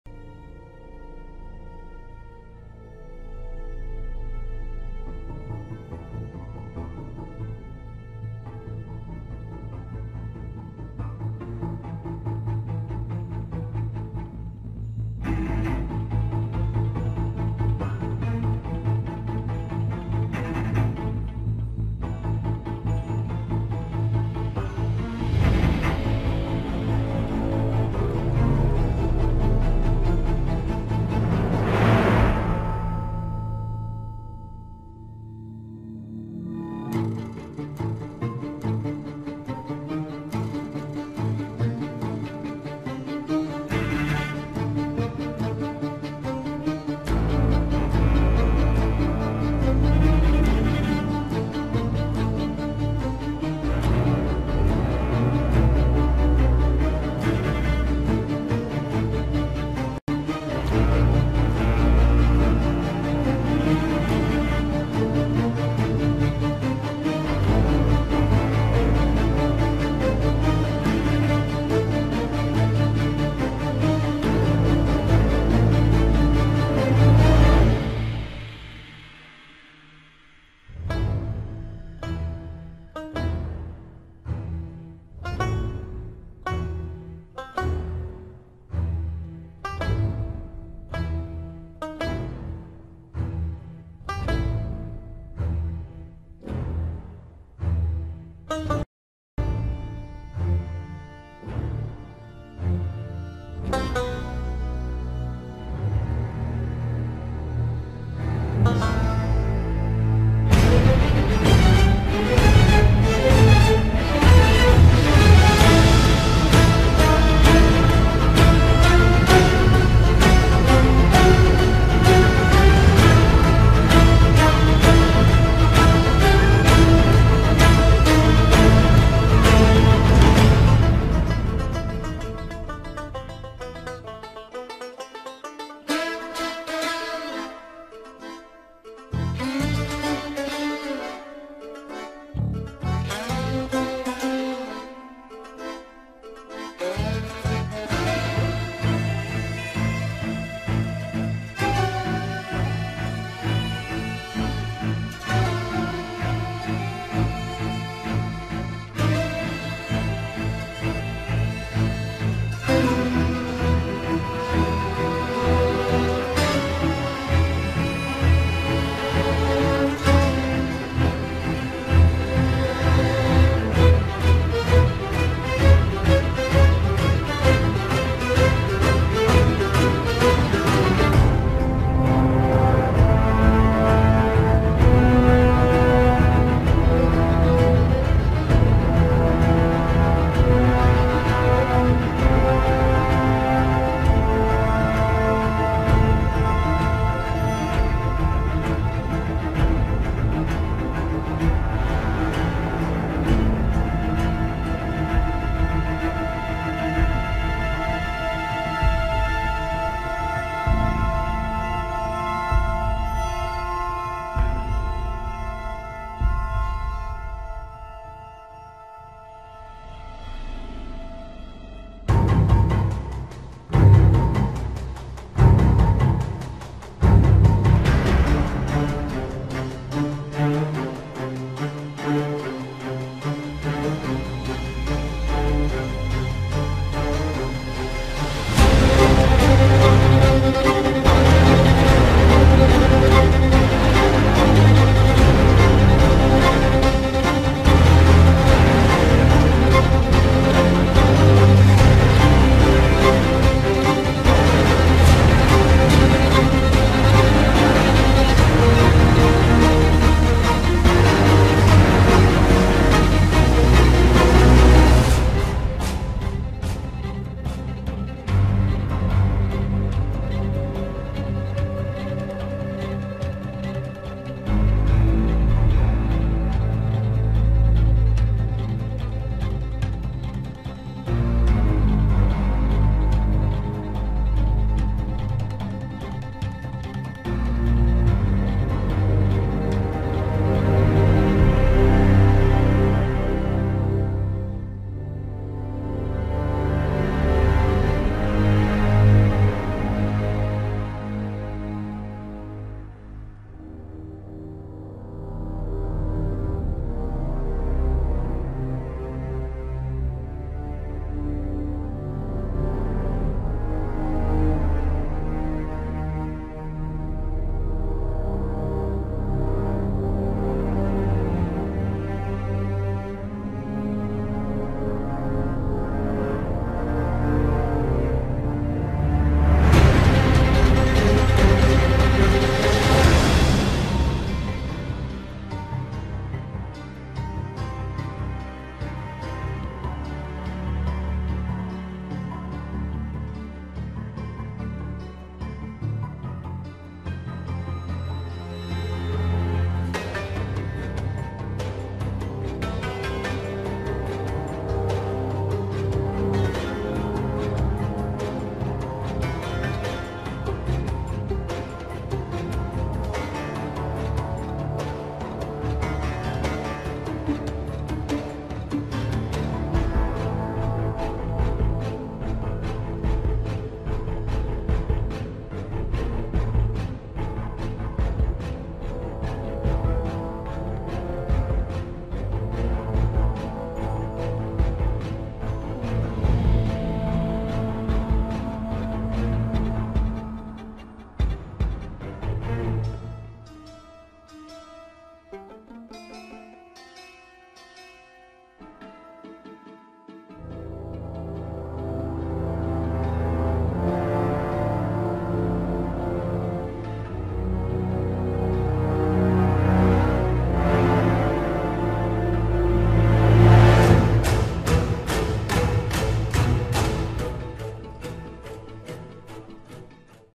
Speeddrawing Sherlocks Holmes and Watson [Full HD]
Here I'm drawing Sherlock Holmes aka Robert Downey Jr. and his friend Watson aka Jude Law. If you enjoy it, please leave a comment or a like ;) That helps me a lot!